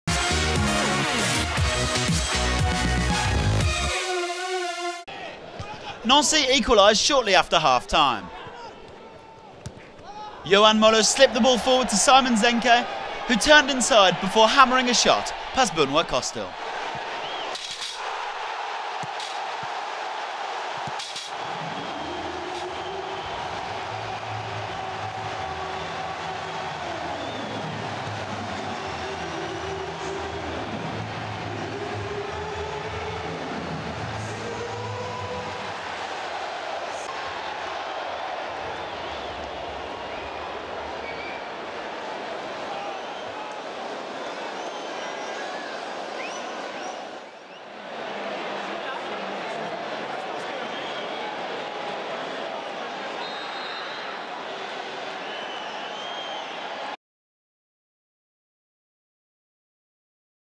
Goal Simon ZENKE (50') - AS Nancy Lorraine - Stade Rennais FC (1-3) / 2012-13
AS Nancy Lorraine vs Stade Rennais FC (1 - 3) : Simon ZENKE (50') goal. All AS Nancy Lorraine vs Stade Rennais FC goals in video.

Stade Marcel Picot - Fri, 09 Nov 2012

Goals : Mevlut ERDING (21' - Stade Rennais FC) / Simon ZENKE (50' - AS Nancy Lorraine) / Romain ALESSANDRINI (74' - Stade Rennais FC) / Jonathan PITROIPA (84' - Stade Rennais FC)